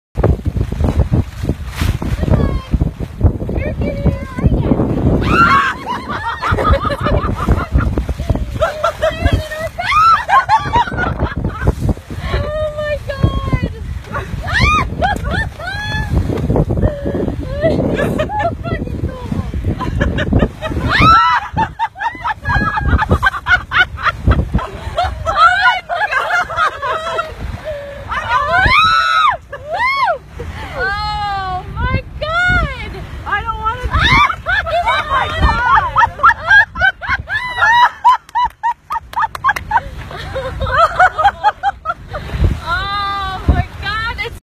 Dolphin Dance on San Francisco Bay
Julia Smith, Marianne Armand  and Debbie Fehr were sailing on the Santana 22 Meliki on San Francisco Bay on July 22 when a pacific bottle nosed dolphin decided to join them and put on a show! This is the 1st document display known of such behavior inside SF Bay and the girls reactions are PRICELESS!!!